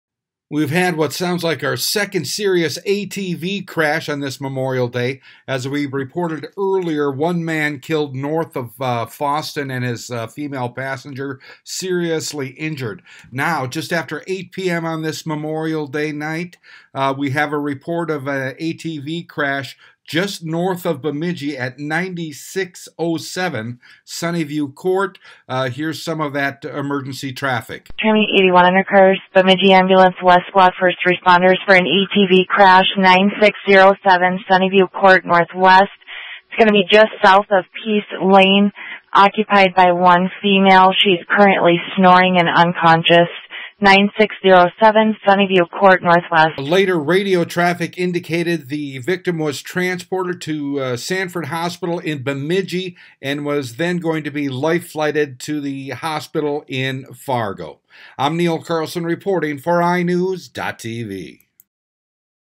Monday Night Report Of Another Serious, Memorial Day, ATV Crash, This 1 Near Bemidji
BEMIDJI, MN (iNewZ.TV) We have a report of a serious ATV crash near Bemidji, Minnesota on this Memorial Day evening. Earlier today, 1 man was killed and a woman injured in an ATV crash near Fosston.